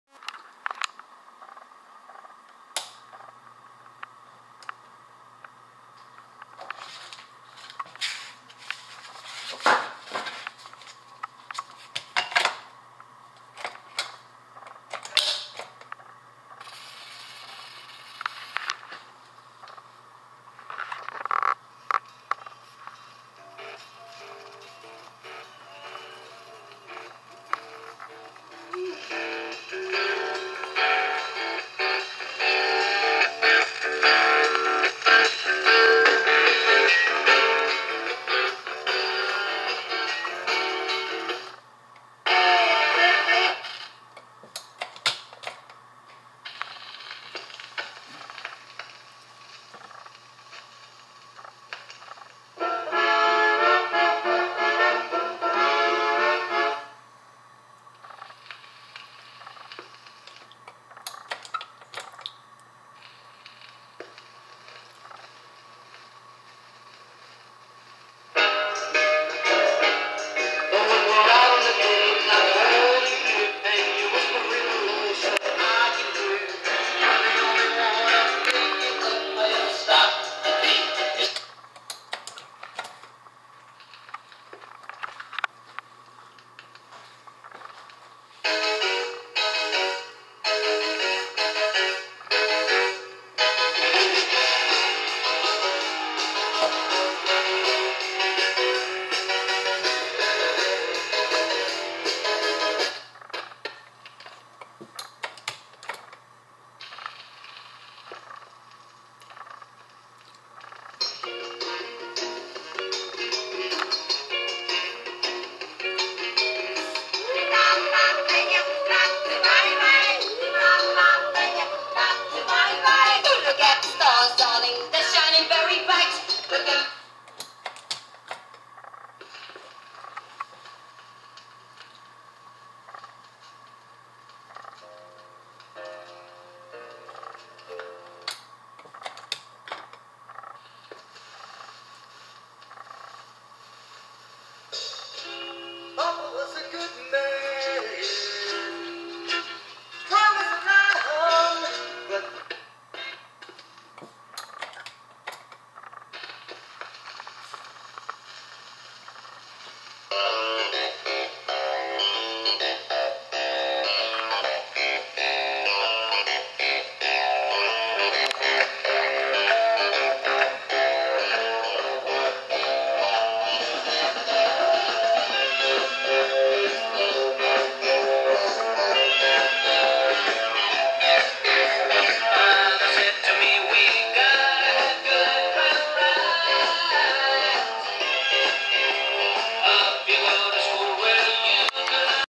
DECCA 45 RPM record player playing a stack of records.
Fully restored Decca record player.  Tube amplified.  EBay listing item is currently for sale.